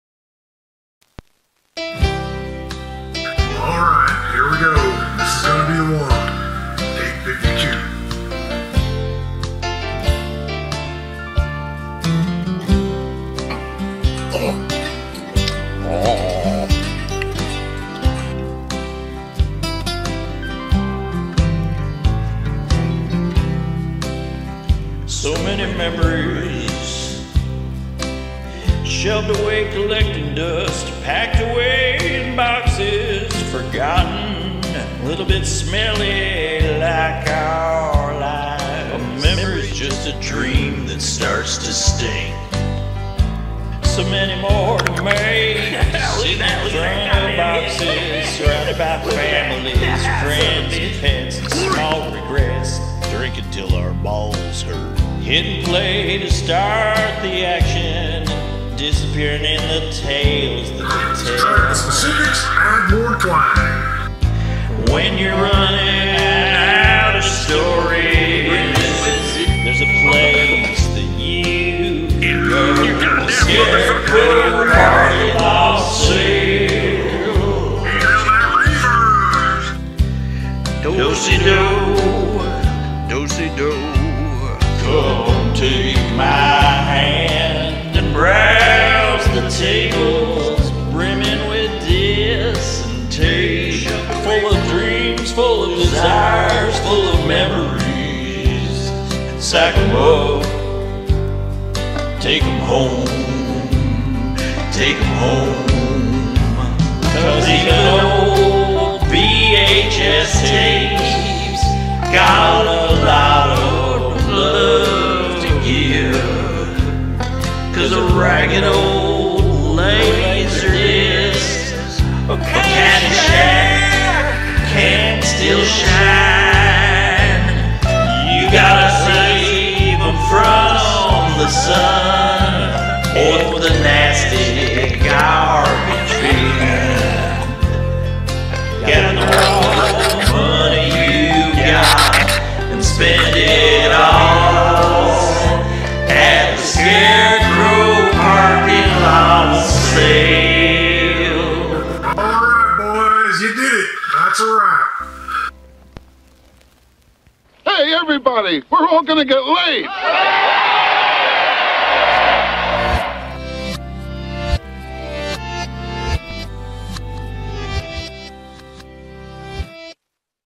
The Scarecrow Parking Lot Sale - 2023
Scarecrow Video's annual parking lot sale returns!
Saturday, July 29, 2023 from Noon-6PM
In the Scarecrow Video Parking Lot at 5030 Roosevelt Wy NE, in Seattle, WA

Come on down for our annual blow-out sale and snag a wide variety of physical media at bargain prices: VHS tapes, Blu-rays, DVDs, Laserdiscs(!) and other film-related goodies. Buy a few things, or buy a bag and fill it with mixed media ($30) or just VHS ($10). DO SI DO!

offer active from 7/28-7/31